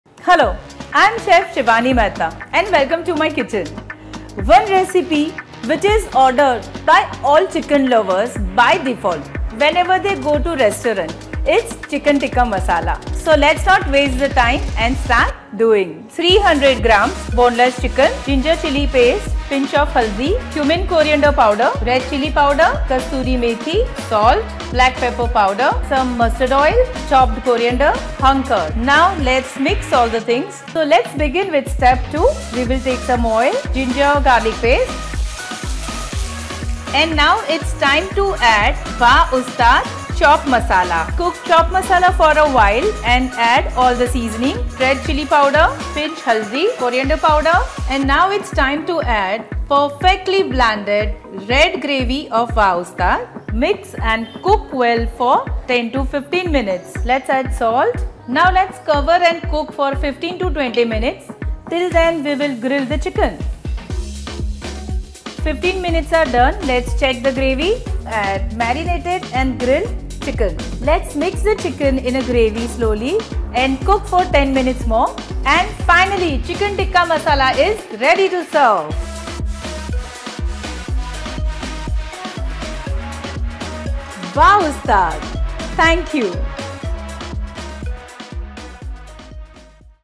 Chicken Tikka With Wah Ustad By Elexa The Studio
Chicken Tikka is a delectable Indian dish that features boneless pieces of chicken marinated in a flavorful yogurt mixture and then grilled or roasted to perfection. The marinade, a symphony of spices like turmeric, cumin, and garam masala, not only tenderizes the chicken but also imparts a tantalizing aroma and a rich, complex flavor profile. The yogurt, a creamy and tangy base, helps to balance the spices and adds a touch of moisture to the chicken, ensuring a succulent and juicy texture. The marinade's spices, including the fiery heat of chili powder and the earthy aroma of ground coriander, create a harmonious blend of flavors that dance on the palate. The yogurt, with its creamy texture and tangy acidity, provides a cooling counterpoint to the spices, preventing the dish from becoming overly spicy. As the marinated chicken is grilled or roasted, the heat caramelizes the exterior, giving it a slightly charred and crispy crust while the interior remains tender and juicy. The result is a dish that is both visually appealing and incredibly flavorful.